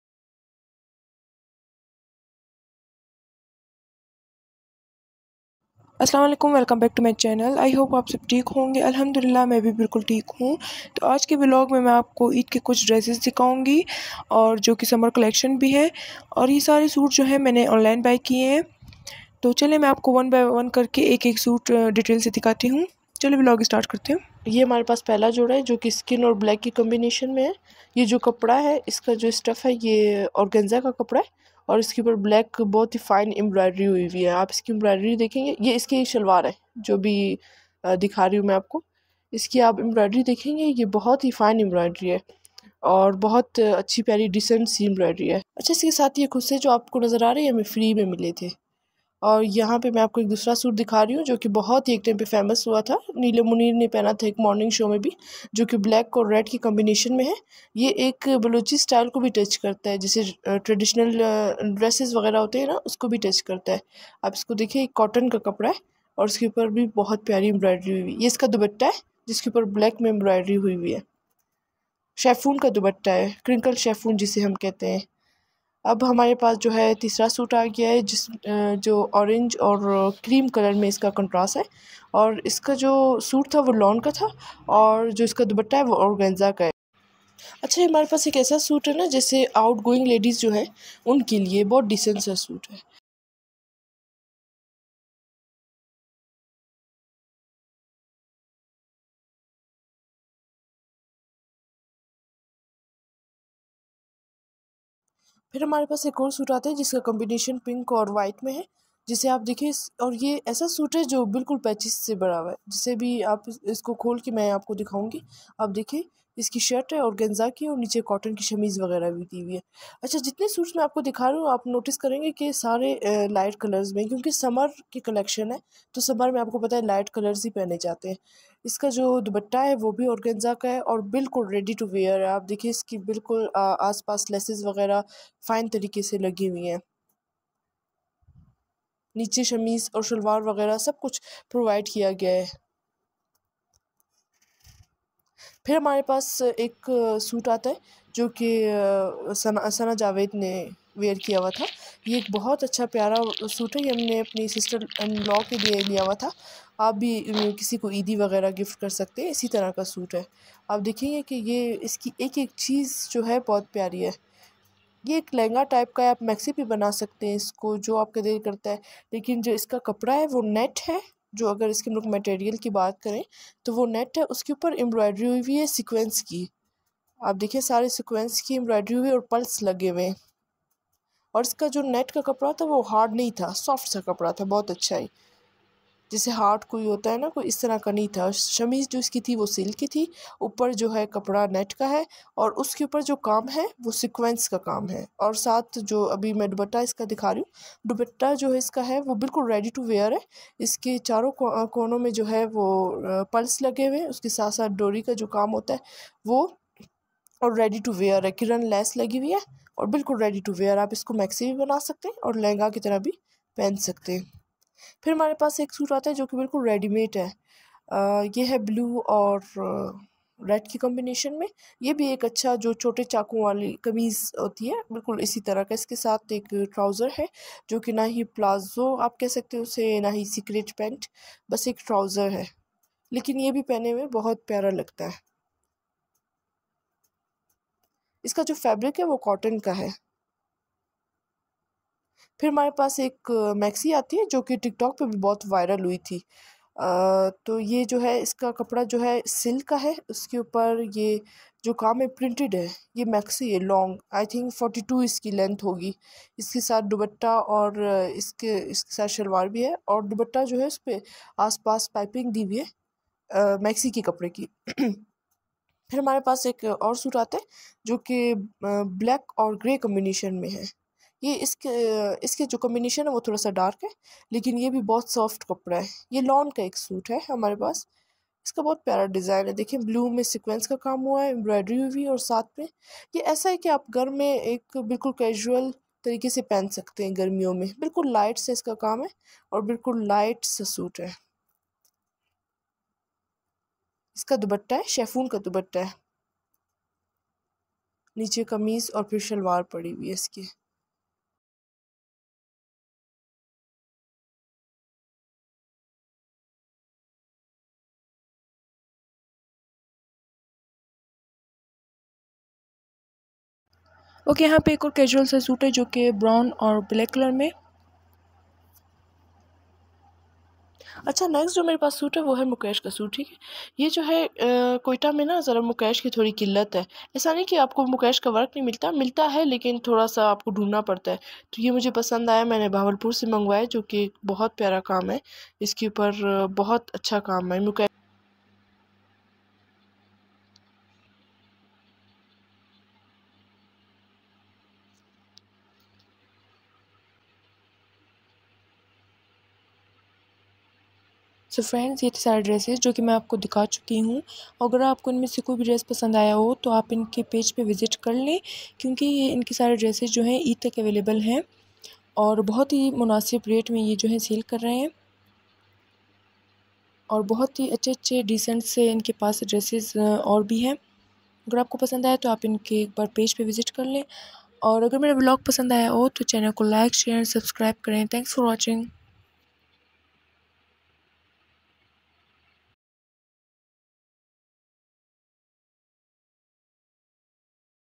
Eid dresses + Summer collection||beautiful dresses#vlog informative Uniworld
here I am showing you my Summer collection!